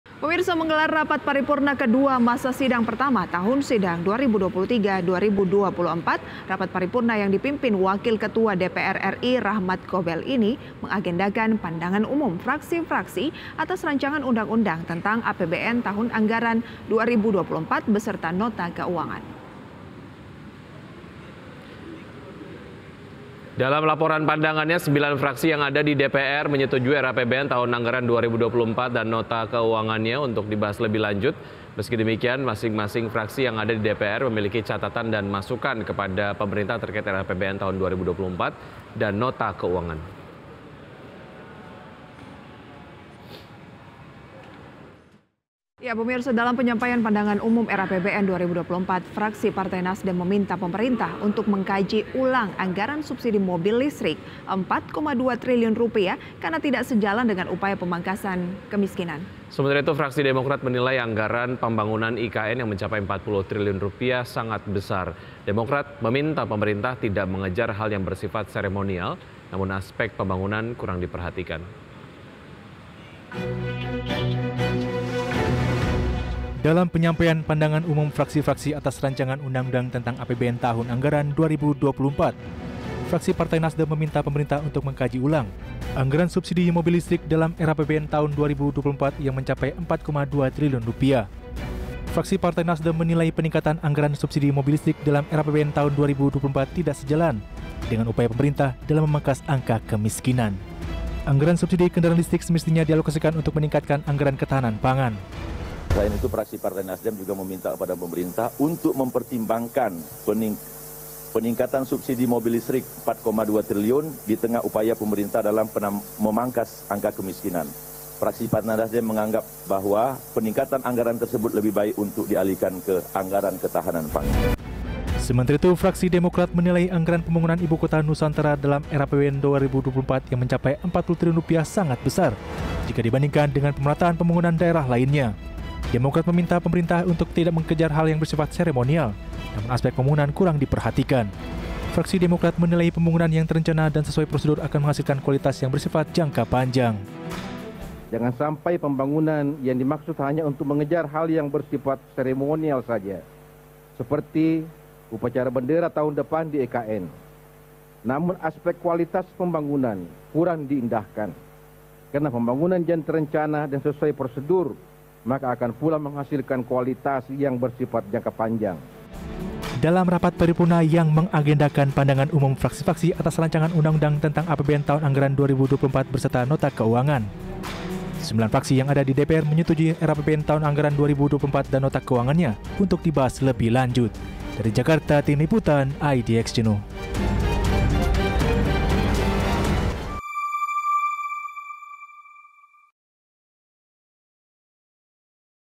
DPR RI Gelar Rapat Paripurna ke-2 | IDX CHANNEL
DPR menggelar Rapat Paripurna ke-2 Masa Sidang I Tahun Sidang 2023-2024. Rapat Paripurna yang dipimpin Wakil Ketua DPR RI Rachmat Gobel ini mengagendakan pandangan umum fraksi-fraksi atas rancangan undang-undang tentang APBN tahun anggaran 2024 beserta nota keuangan.